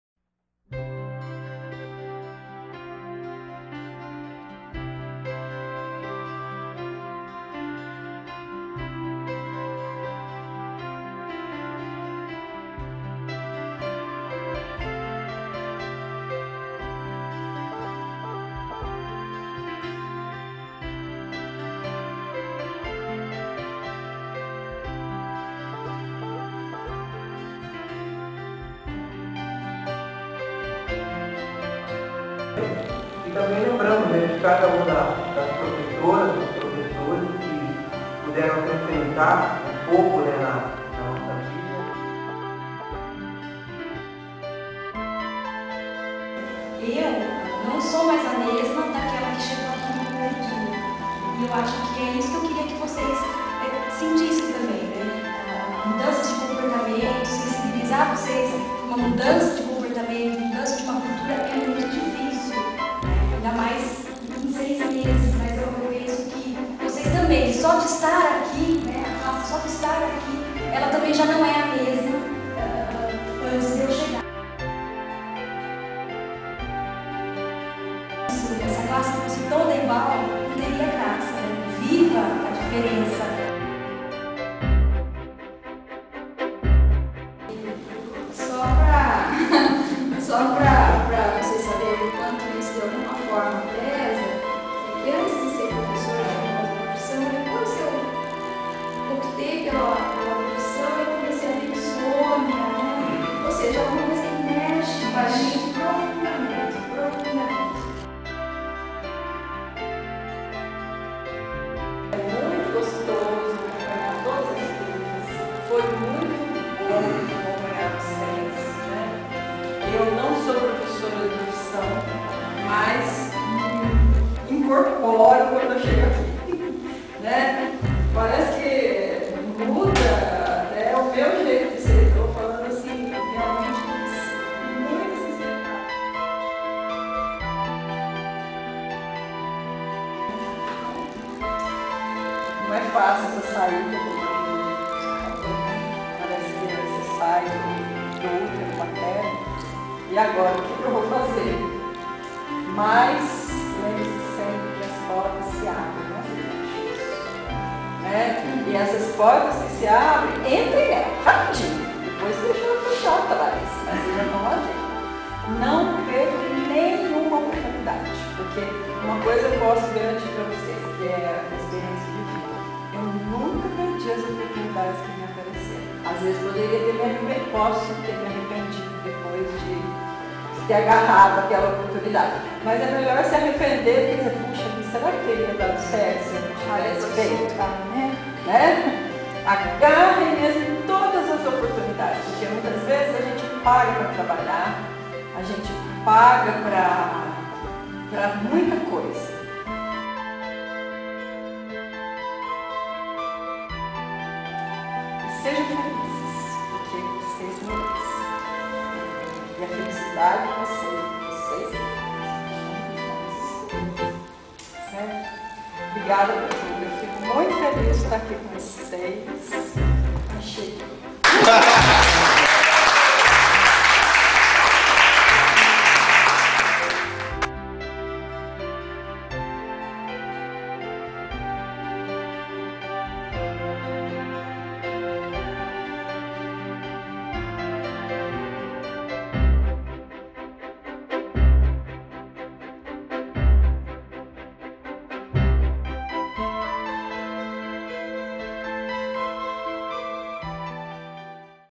docentes - missão cumprida / Saneamento Ambiental - FHO UNIARARAS
As palavras das professoras do curso de Gestão e Saneamento  Ambiental na confraternização com os alunos do 3º ano no último dia do curso.
Professoras: Aurora, Roselene e Júlia.
Lembranças da 3ºano Gestão Ambiental
UNIARARAS - (Turma 2008)
Comemorar é trazer a memória um acontecimento. Celebrar é demonstrar sentimentos com euforia. Festar é o ato de celebrar uma comemoração. Comemorar com ou sem ritos e cerimoniais é regra do bem viver. Portanto, vamos comemorar todos os acontecimentos que fazem história e permeiam as memórias de forma agradável.